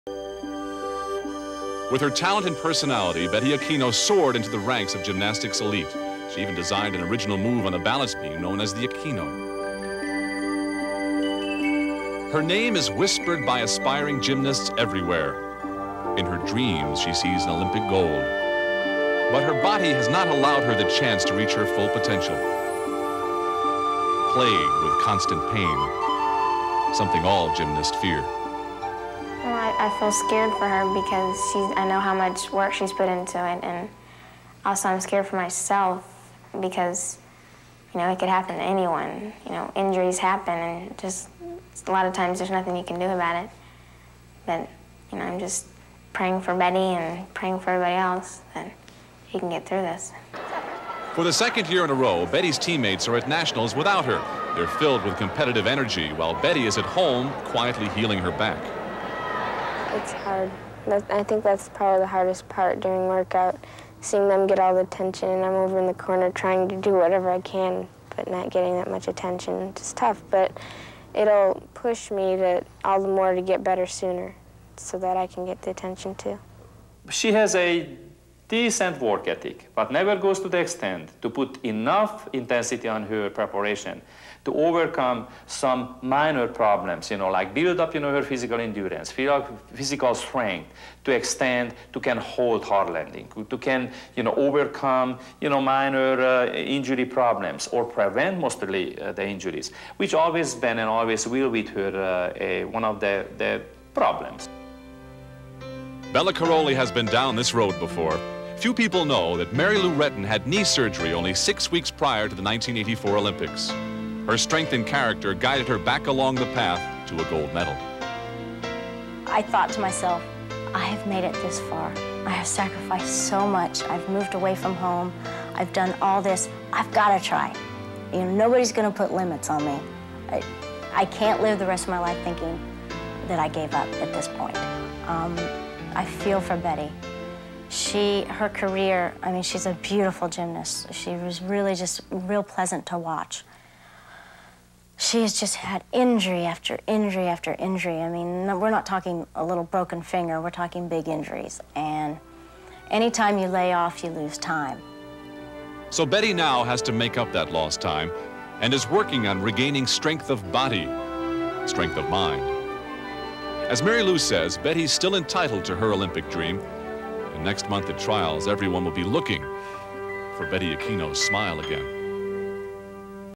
Vignette - 1992 Phar-Mor U.S. Championships - Women
May 16, 1992 - St. John Arena - Columbus, Ohio
Gymnastics
Betty Okino, Kim Zmeskel, Bela Karolyi, Mary Lou Retton